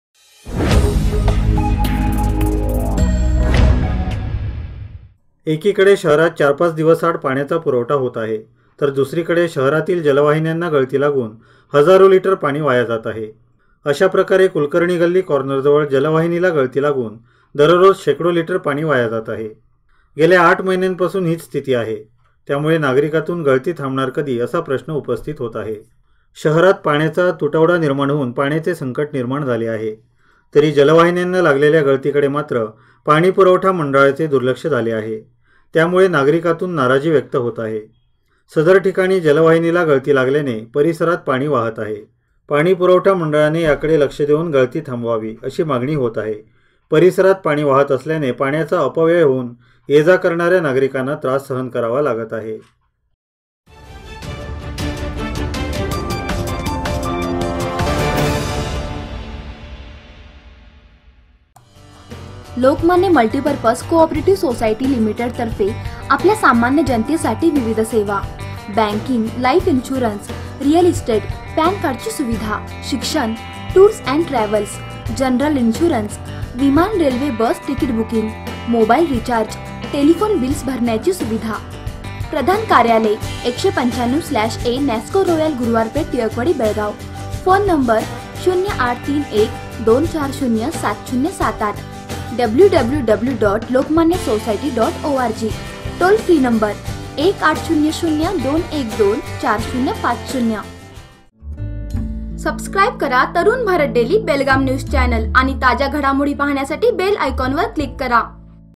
गळती थांबणार कधी ? | Belgaum News | 01-04-2019
|Tarun Bharat Social Media | तरुण भारत | water leakage | Belgaum |
एकीकडे शहरात चार-पाच दिवसाआड पाण्याचा पुरवठा होत आहे. तर दुसरीकडे शहरातील जलवाहिन्यांना गळती लागून हजारो लिटर पाणी वाया जात आहे.